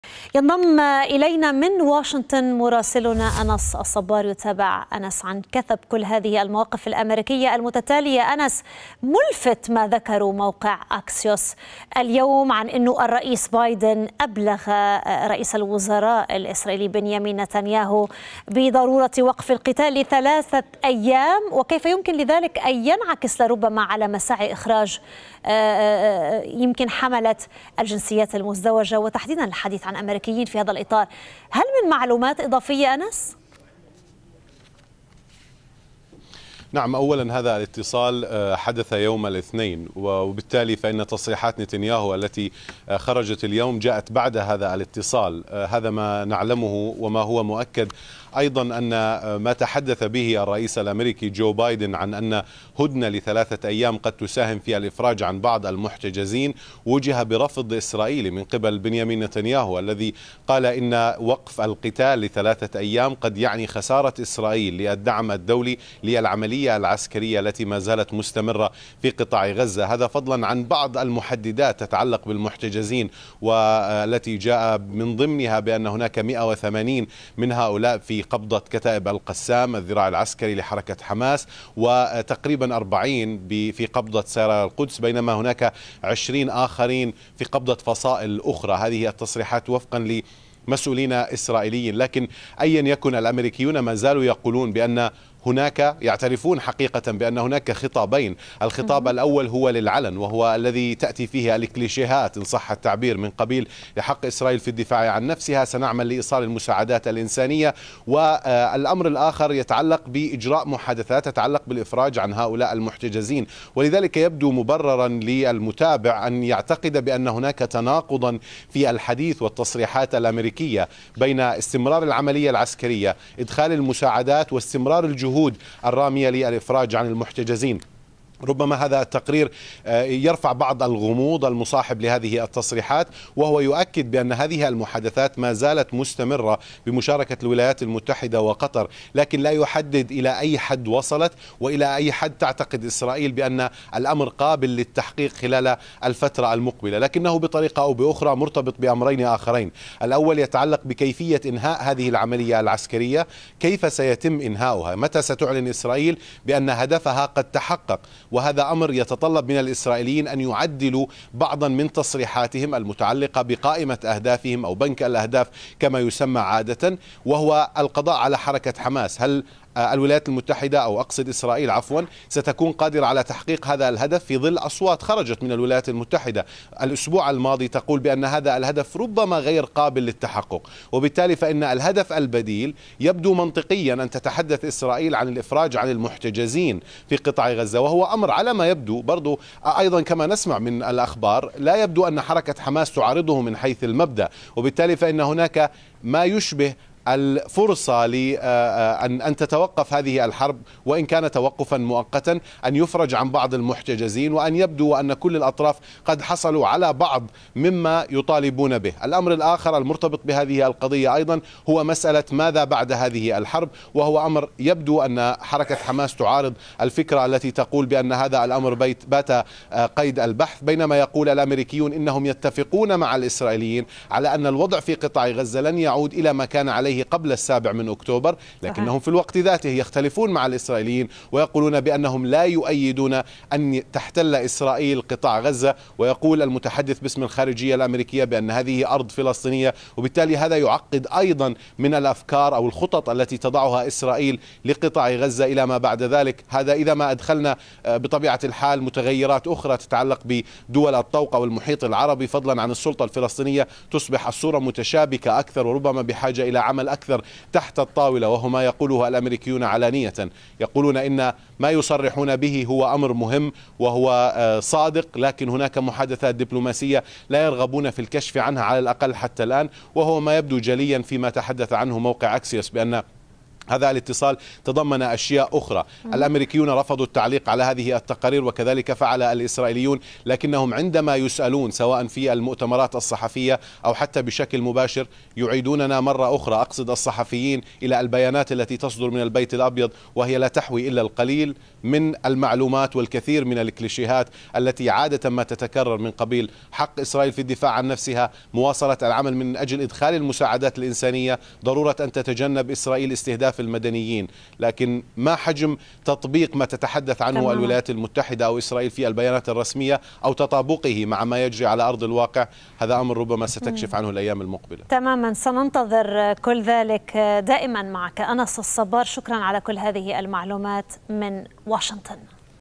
البيت الأبيض: حان الوقت لمواصلة السعي لتوقف في القتال في قطاع غزة من أجل أغراض محددة
هذا وأكد المتحدث باسم الخارجية الأميركية فيدانت باتيل معارضة الولايات المتحدة احتلالا جديدا طويل الأمد لقطاع غزة من جانب إسرائيل
وأضاف باتيل أن واشنطن ترفض أي تهجير قسري لسكان القطاع خارج أراضيهم مؤكدا على أن غزة هي أرض فلسطينية وستبقى كذلك.
وشدد المتحدث الأميركي على أن الأمور لن تعود الى الوضع الذي كان قائما في السادس من أكتوبر في إشارة إلى اليوم الذي سبق هجوم حماس.